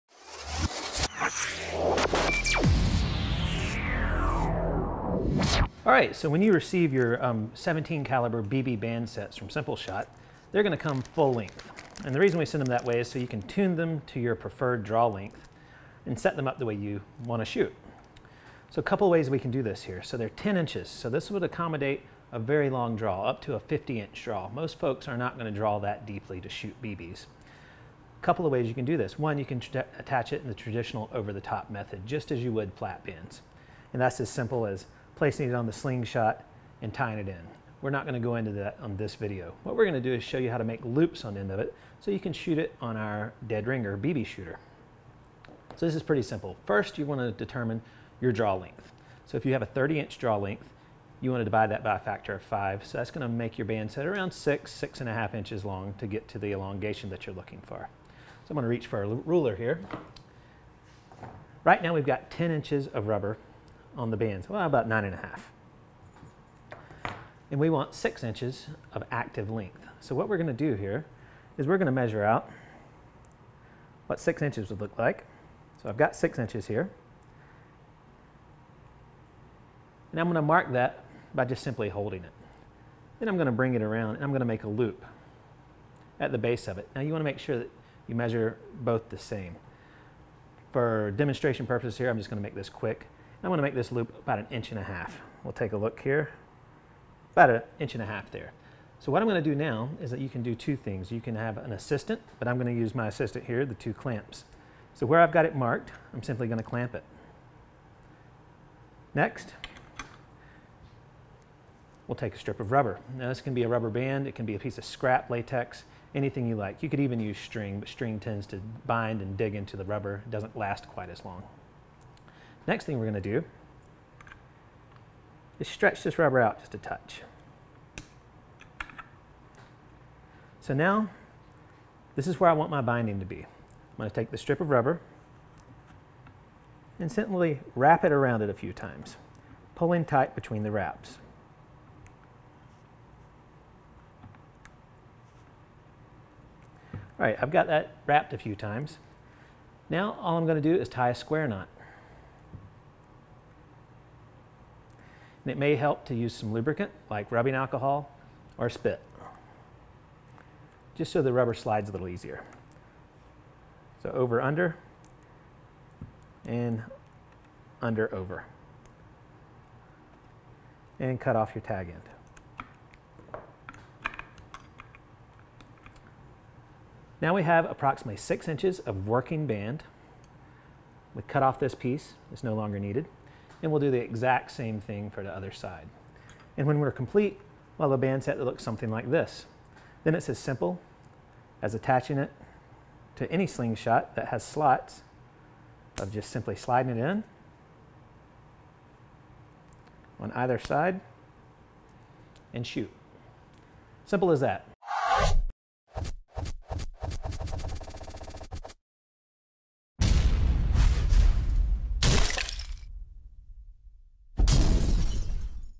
SimpleShot BB Bands
The tubular BB bandsets offered at SimpleShot can be shot a variety of ways.  Here we show you how to set them up as pseudo tapers.